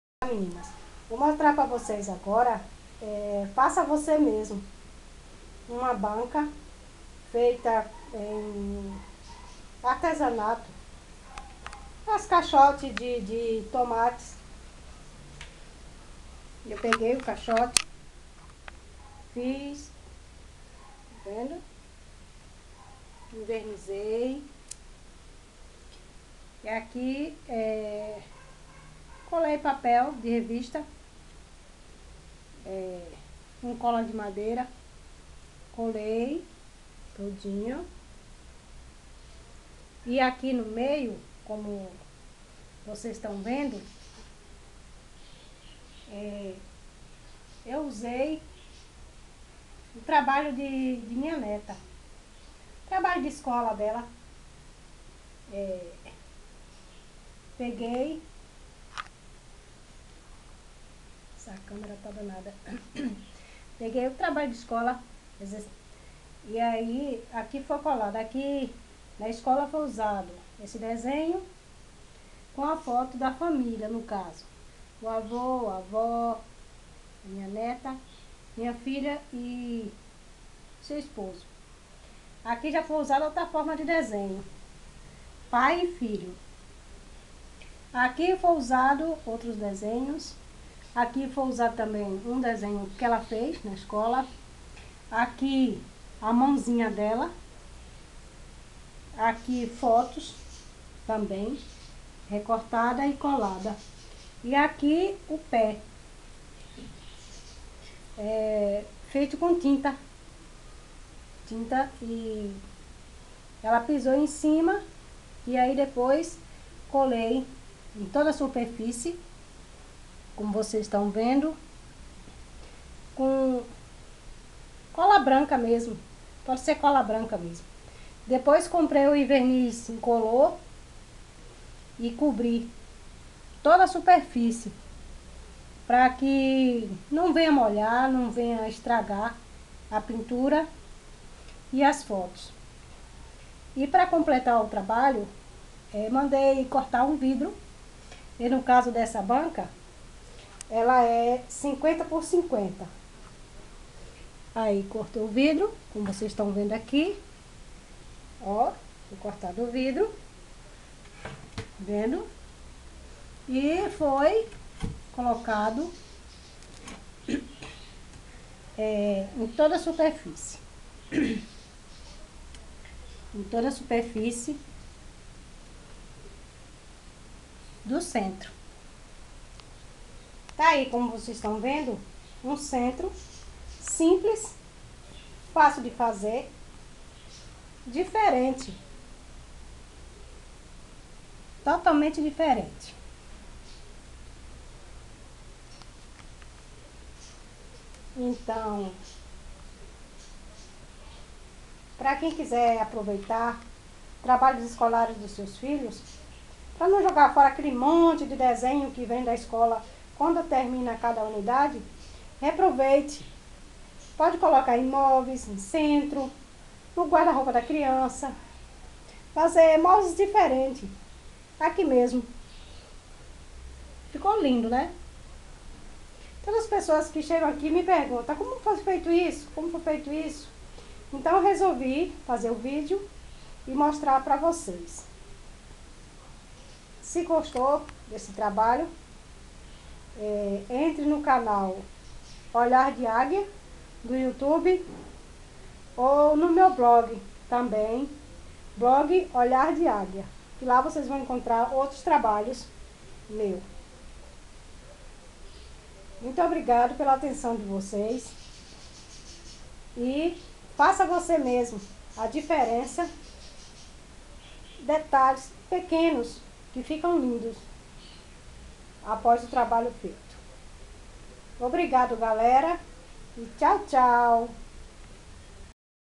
APRENDA COMO FAZER : Um centro sala com caixotes e trabalhos escolares do seu filho.
Descrição:   Caixote de tomate, trabalho escolar, cola branca , verniz incolor , verniz cor tabaco ,  papel de revistas , vidros do tamanho  50 x 50 .